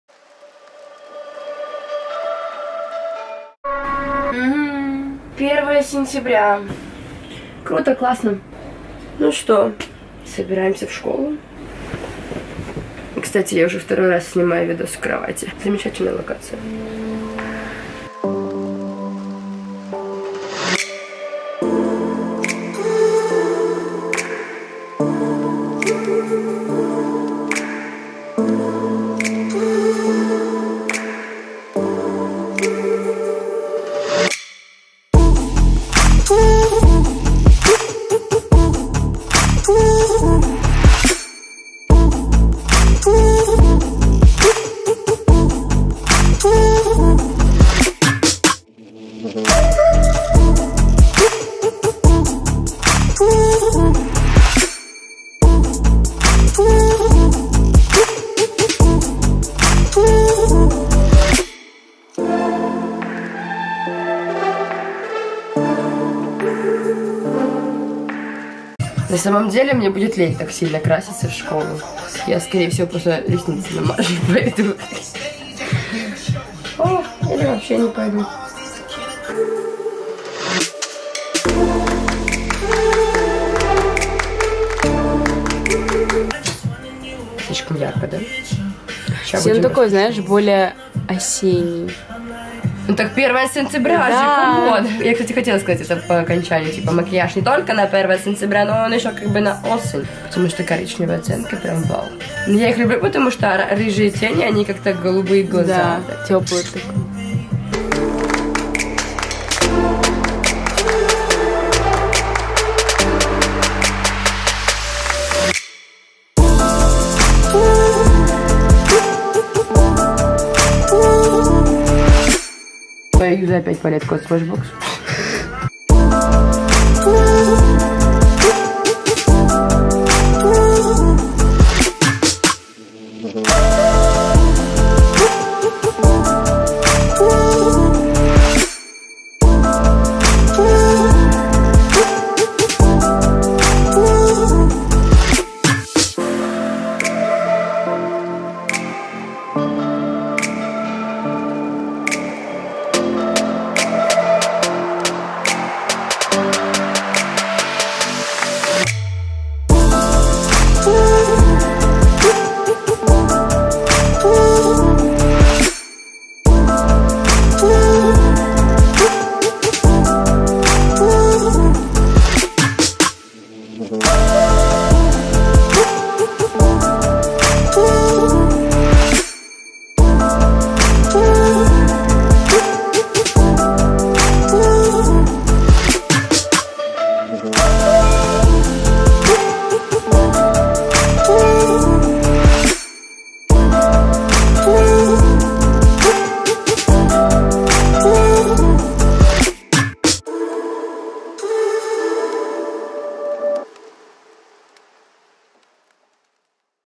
Back to school || 1 сентября || Get ready with me ♡
1 сентября на носу, сегодня мы с вами соберемся на сие мероприятие) Макияж+ лук!

Книга _Special_dates_

-ссылки-

♡Periscope Гудменс
По всем вопросам: